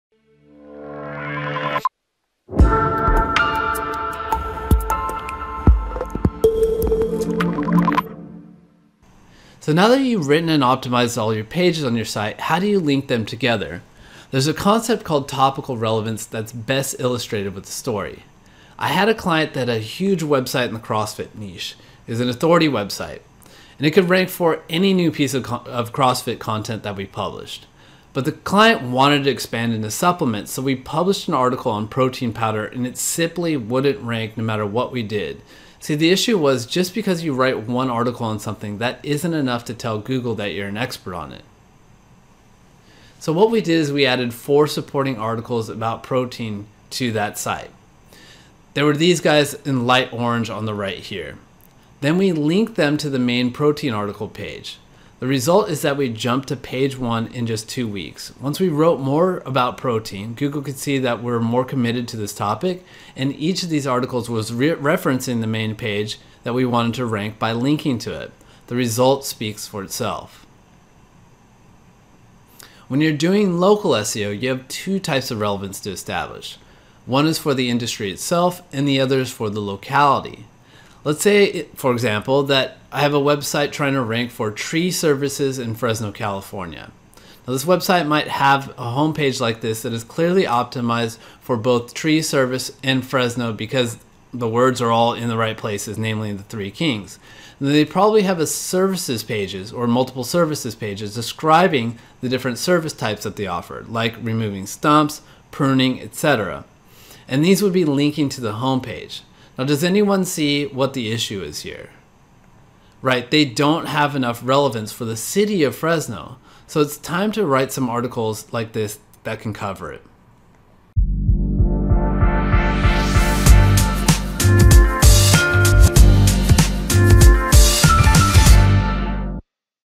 How to Interlink your Pages Together - SEO Beginner's Guide [Part 5]
Beginner SEO Series Episode 5

After you've created the various pieces of content on your site, you need to link the pages together.

When you link from one page to another, it helps to establish what is known as "relevance".

Learn more SEO at...